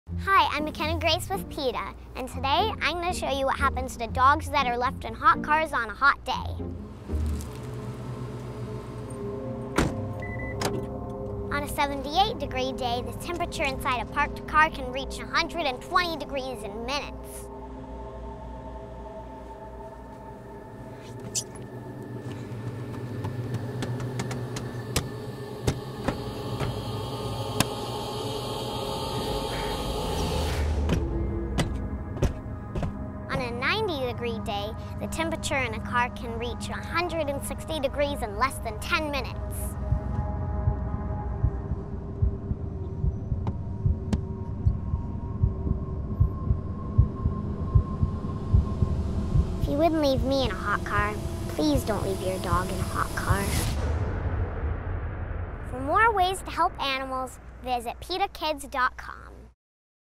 Mckenna Grace Gets Trapped in a Hot Car
Actor Mckenna Grace shows the pain that a dog goes through when locked inside a car during even a mild summer day.